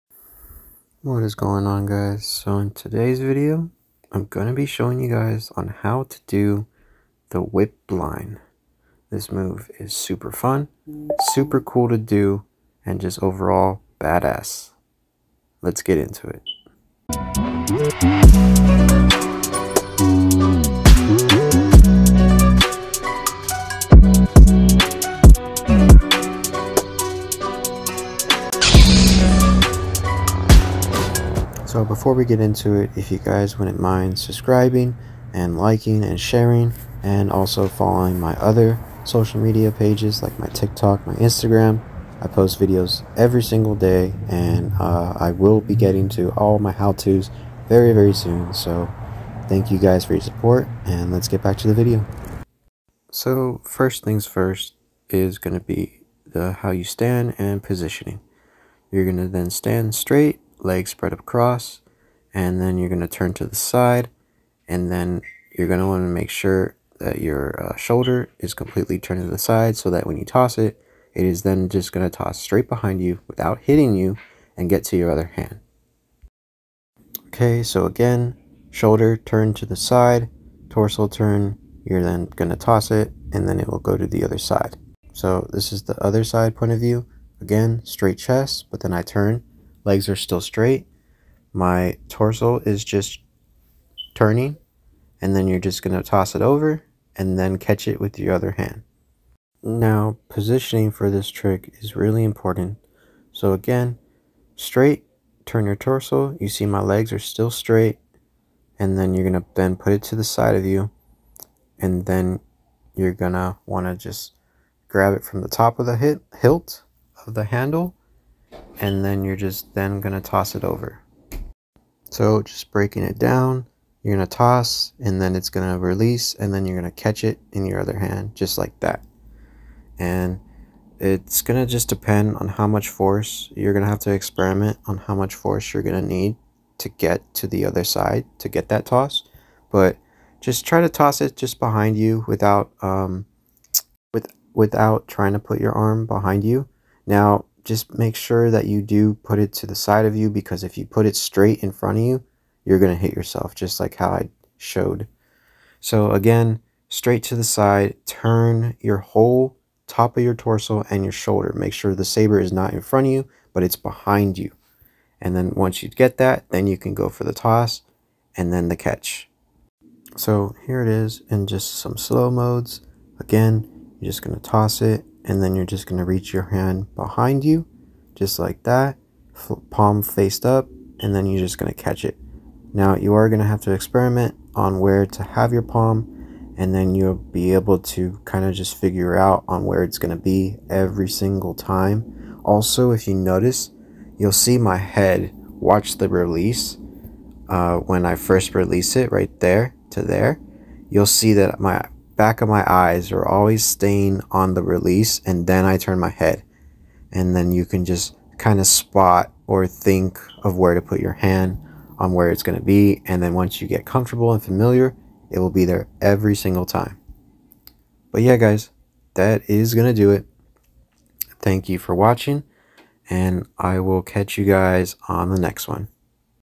How to do the Whip Blind, with a Lightsaber!
Hey guys, I hope this video helps you guys learn on how to do this Spin. To  either help a beginner to  get into spinning or to get you to  your goals to either Spin like I do or try to  surpass me. Either way, you will reach whatever it is you're trying to accomplish! 😇 best of luck!! I also linked another how to of this back in the day.